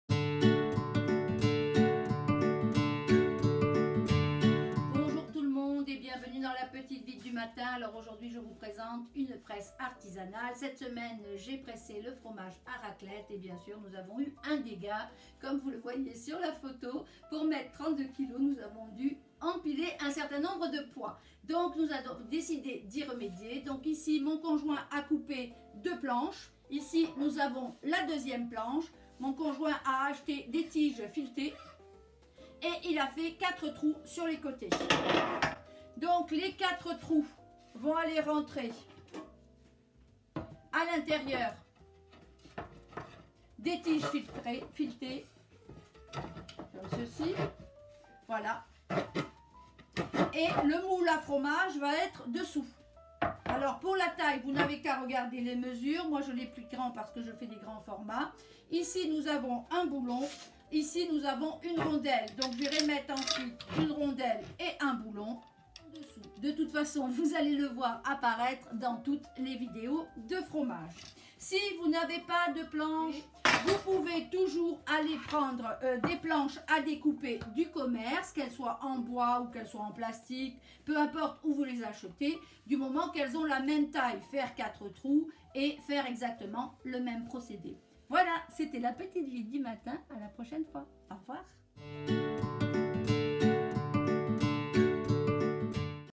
Comment faire une presse à fromage maison, les petites vites
Ca reste assez simple et surtout pratique pour les fromages à pâtes pressées.